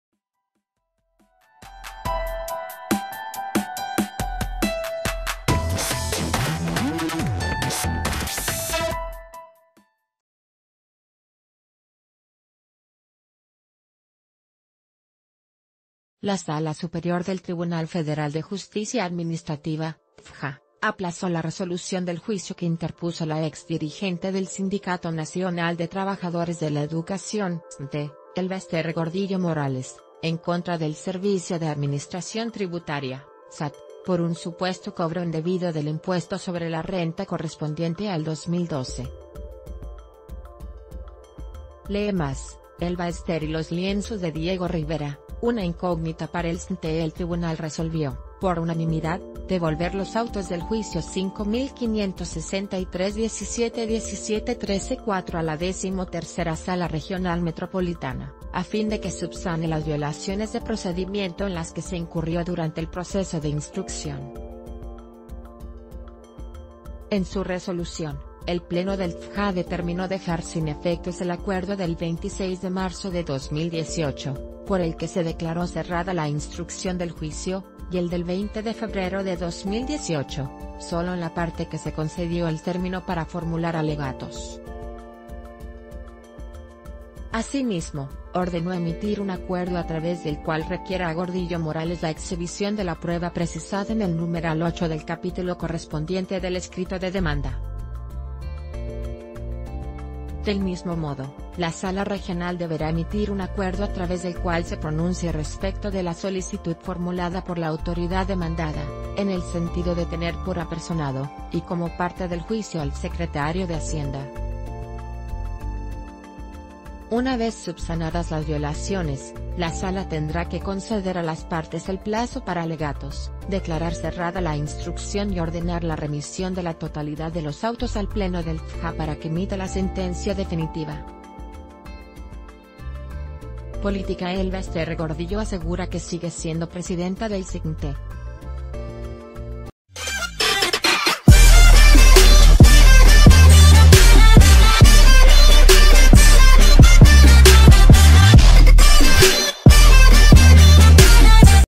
Tribunal aplaza resolución de juicio de Elba Esther contra el SAT
La Sala Superior del Tribunal Federal de Justicia Administrativa (TFJA) aplazó la resolución del juicio que interpuso la ex dirigente del Sindicato Nacional de Trabajadores de la Educación (SNTE), Elba Esther Gordillo Morales, en contra del Servicio de Administración Tributaria (SAT) por un supuesto...